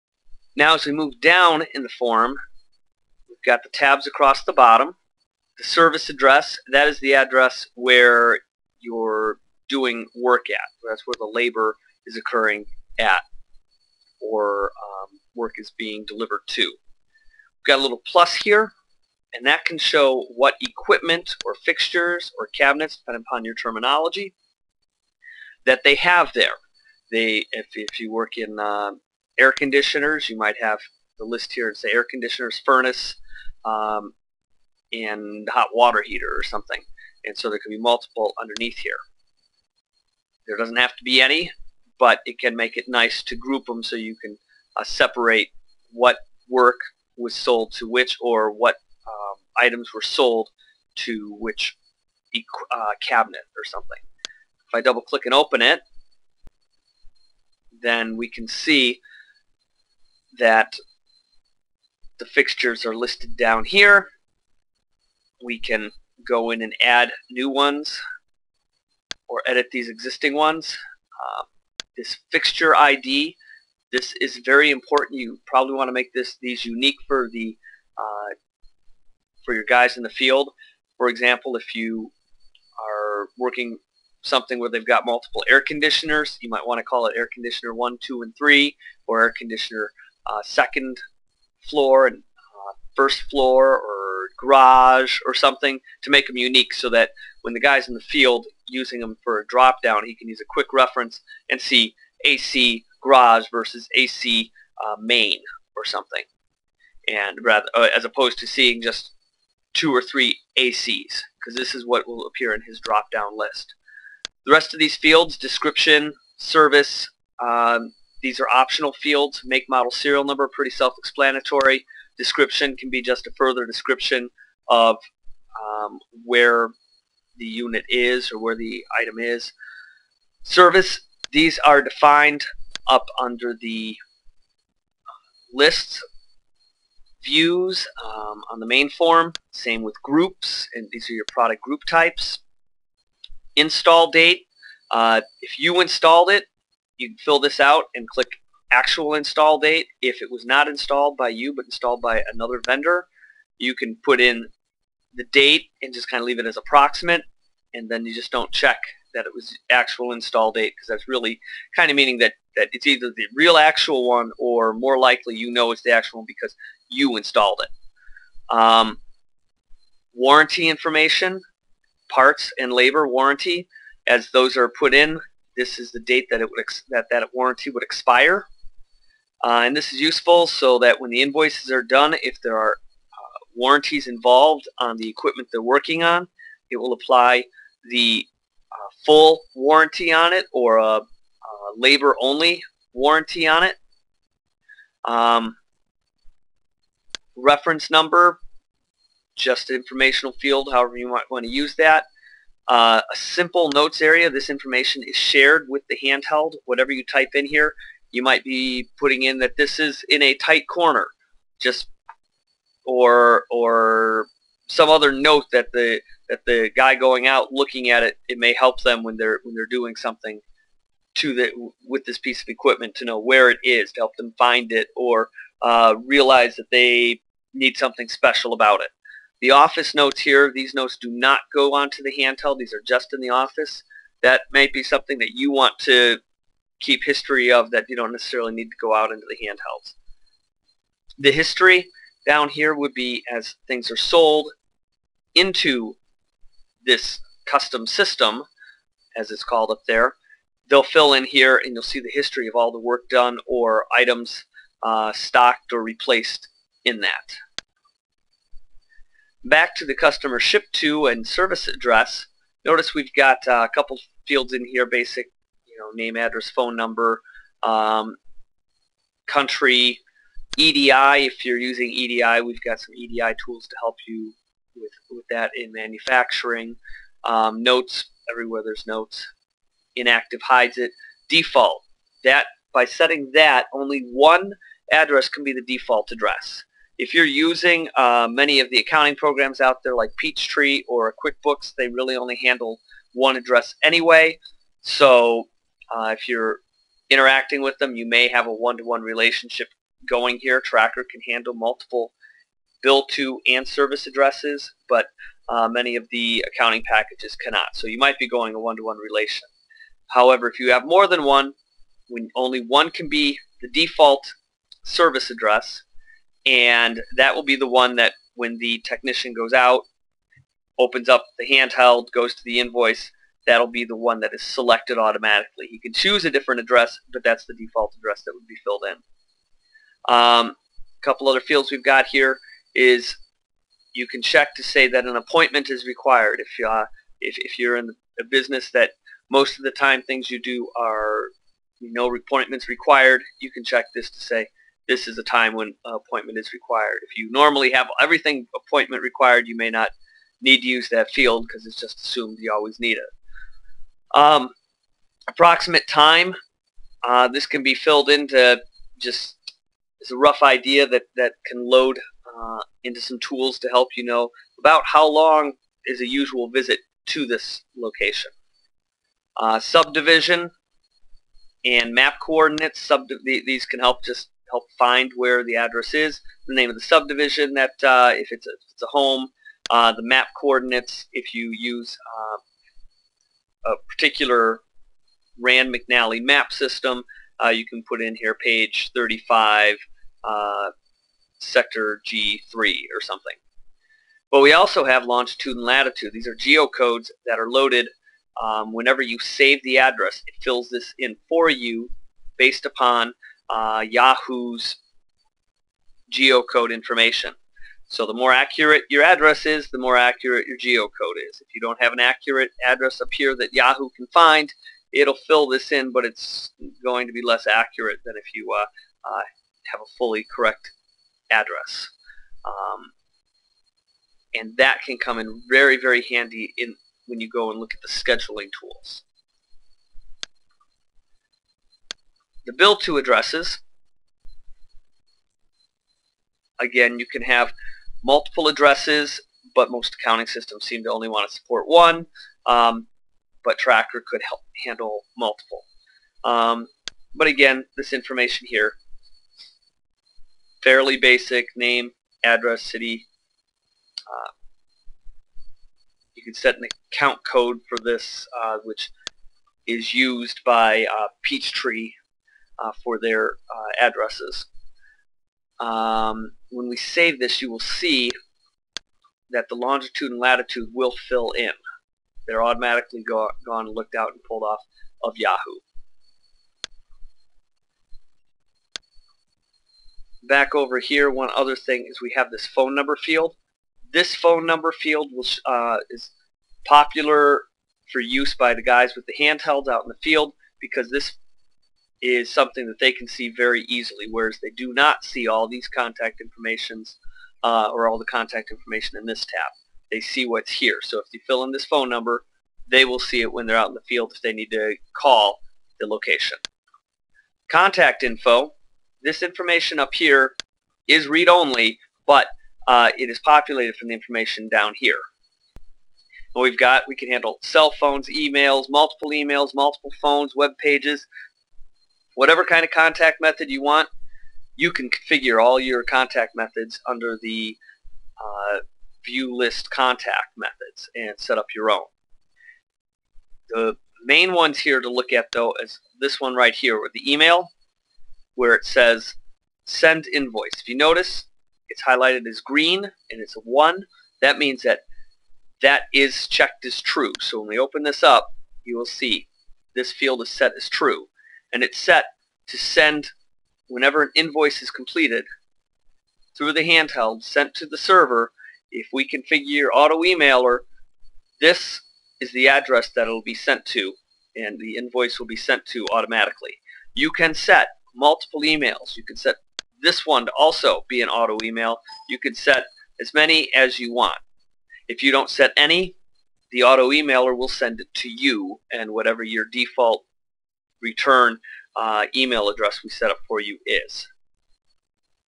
Customer Overview Part 1
Trakker Customer Data Overview Part 1.
Learn more about our Inventory Control, Invoicing and Scheduling and Dispatching Software